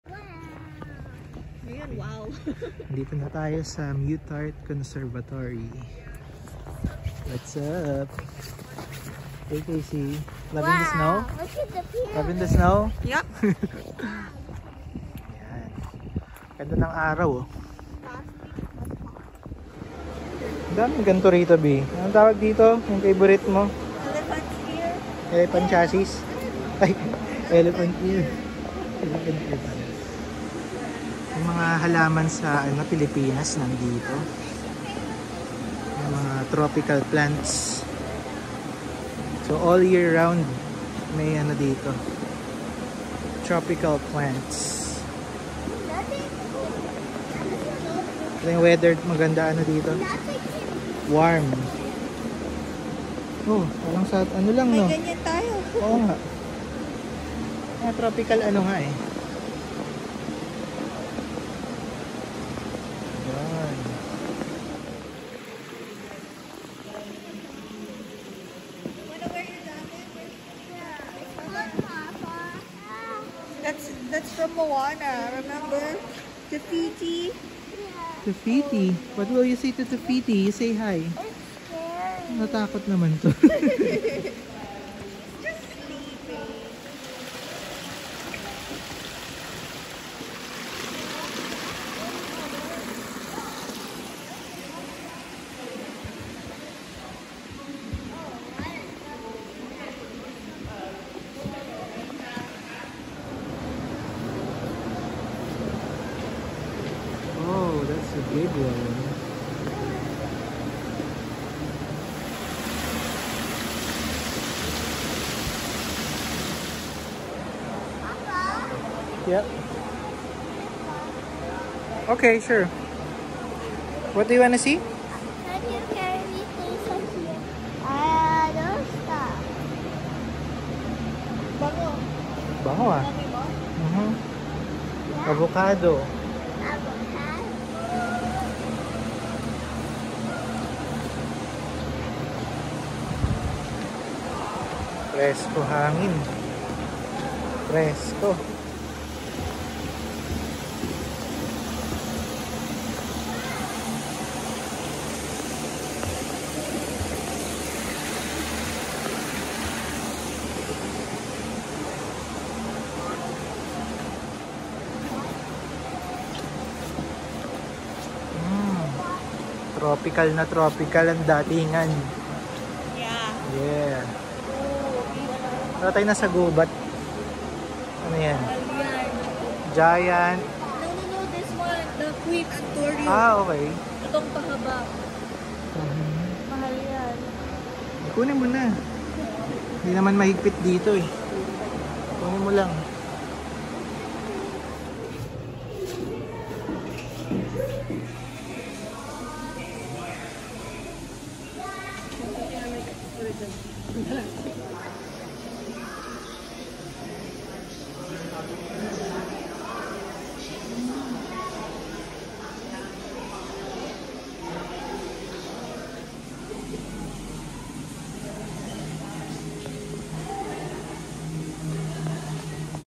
Muttart Conservatory - Edmonton, AB Canada - January 2023
Last January 2, 2023 we visited the Muttart Conservatory in Edmonton, Alberta, Canada. In this video, we'll show you around the conservatory and give you a glimpse of the beautiful plants and flowers we saw. It's been like 7 years since I've been here, and they are open all year round. Perfect quick getaway from the harsh cold of Albertan weather.

Overview:
The Muttart Conservatory is part of Edmonton's River Valley Parks system, which is the largest stretch of urban parkland in North America, covering over 7,400 hectares.

It is a botanical garden located in the heart of Edmonton. It features four glass pyramids that house different plant ecosystems, including arid, temperate, tropical, and a feature biome that changes periodically. The pyramids are made of 3,000 panes of glass and rise 20 meters above ground.

It's been  featured in several movies and TV shows, including the 1993 film "Cool Runnings" and the TV series "The X-Files."

Muttart Conservatory
Botanical Garden
plants s
flowers s
pyramid s